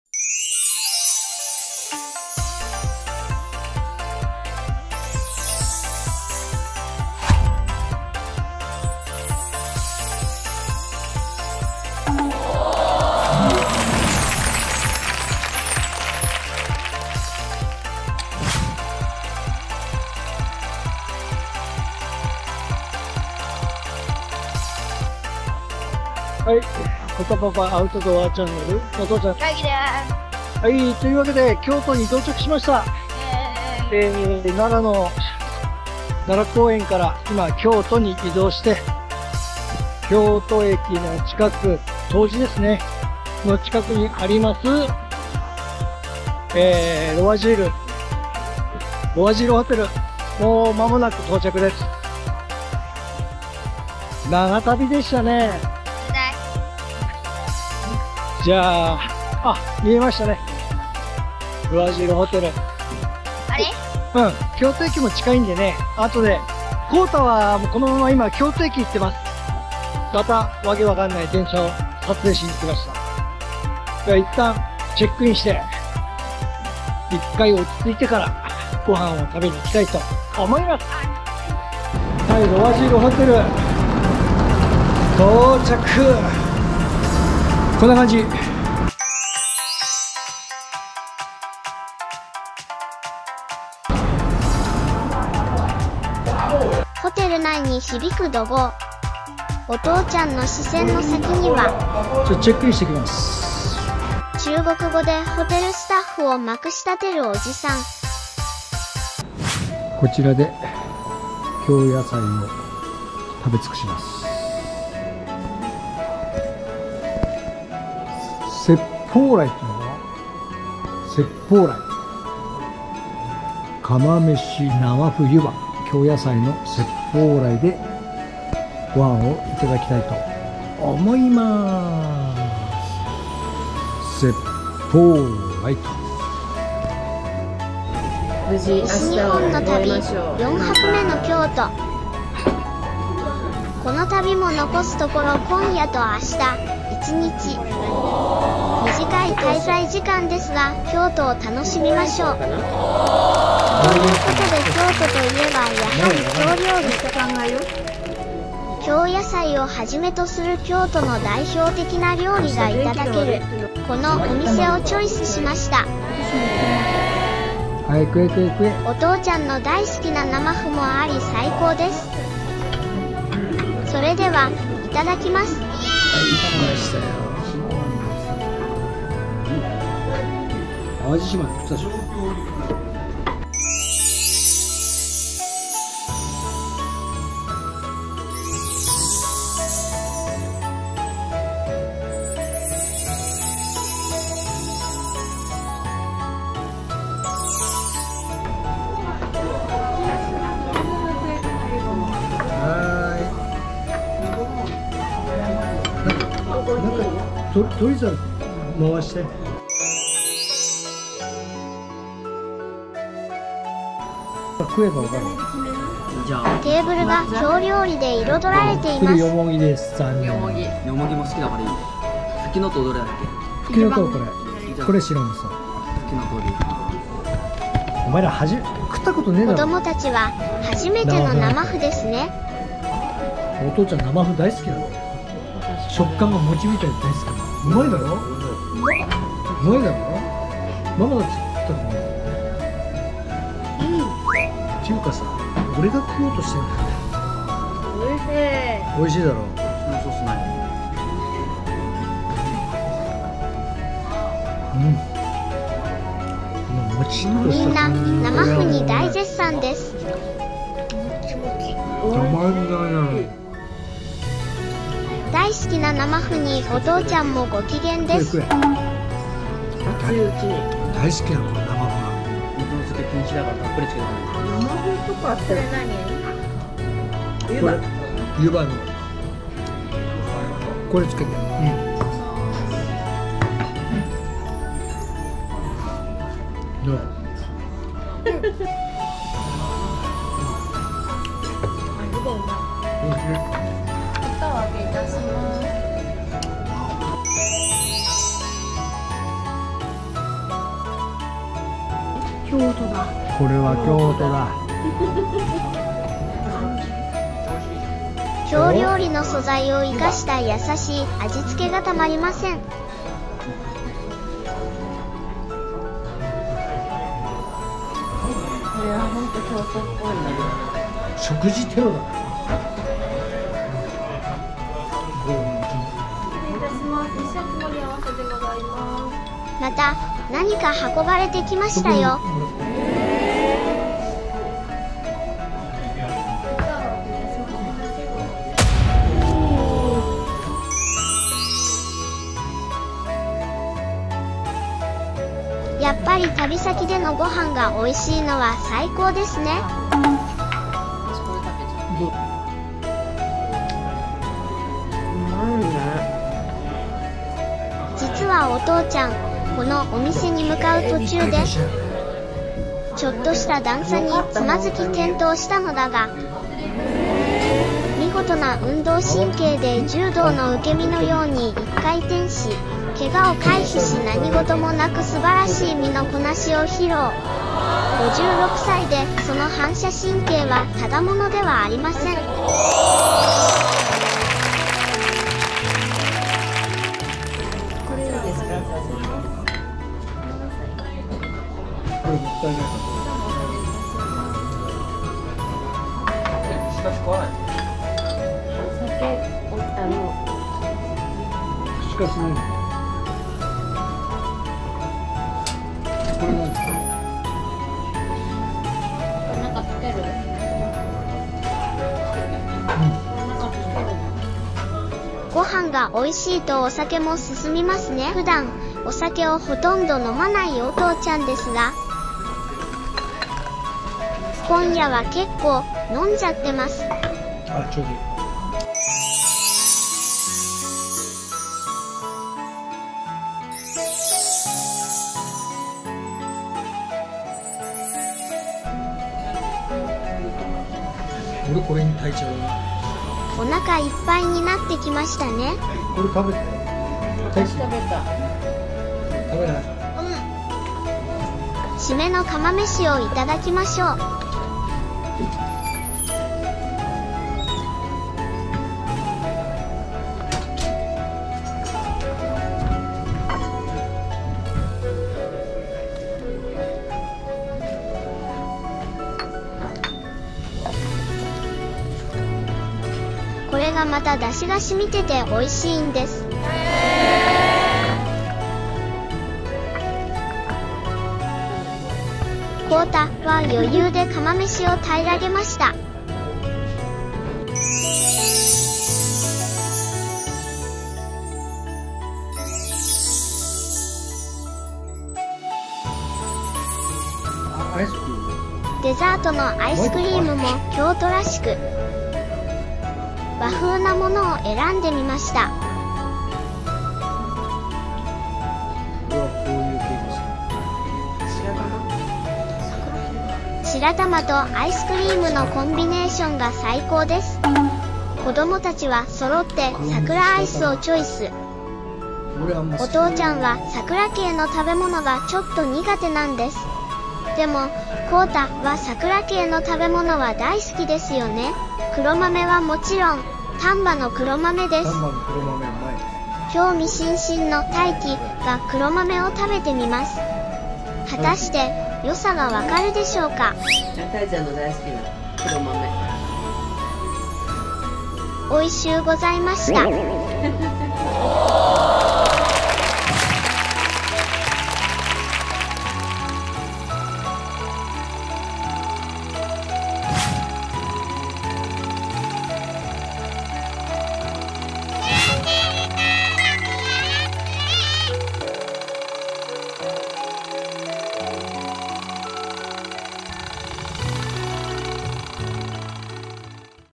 【家族旅行】西日本の旅 京都の夜を京料理で彩る～第16章 京やさい料理 接法来～【京都】
西日本の旅もいよいよ大詰め！
最後の訪問地、京都に到着しました！
京都と言えば京料理、と言うことでお父ちゃんが選んだお店は
京都駅ビルにある京やさい料理 接方来。
京野菜をはじめとする京都の代表的な食材を使った料理が頂けます！
京野菜・生麩・湯葉・釜めしなどなど
滞在時間は短いですが食で京都を堪能しよう！